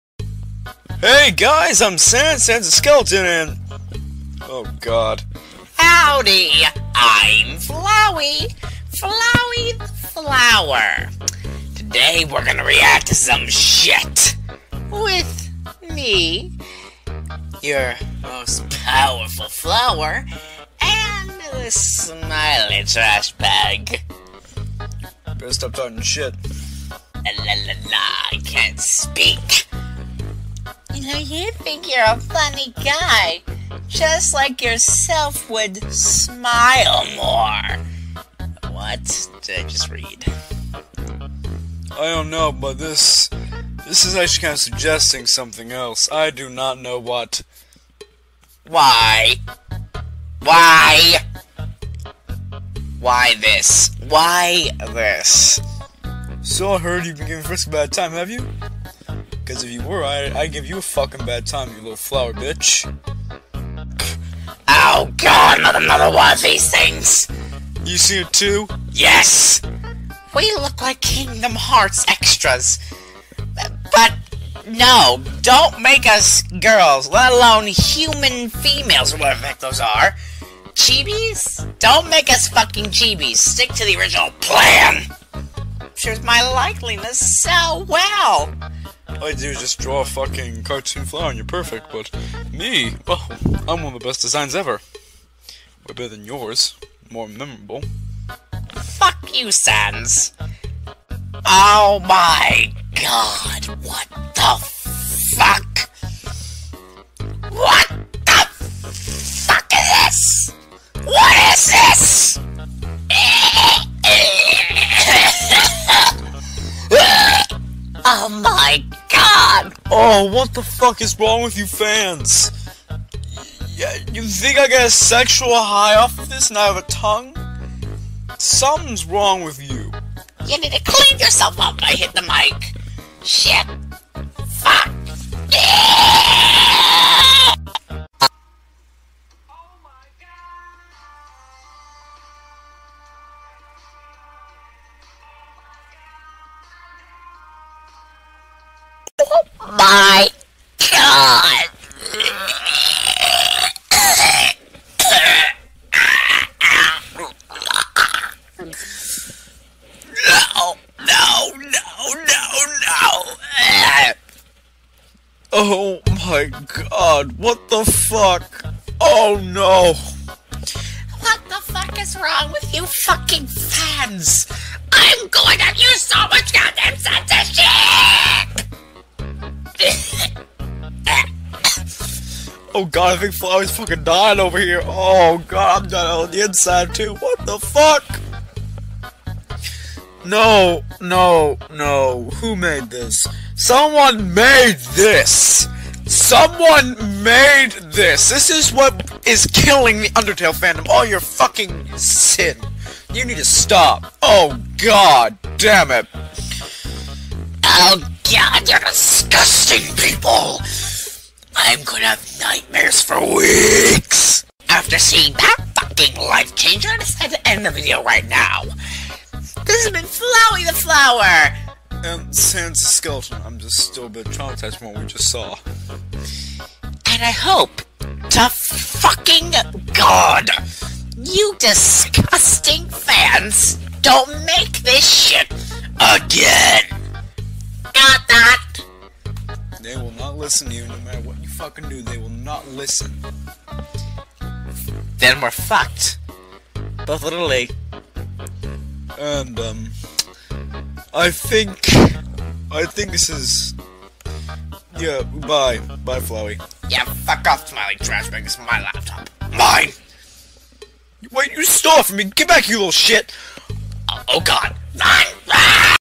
Sans and Flowey react to Undertale fanart
OMG WHHHHHHYYYY
audio taken from Jon Tron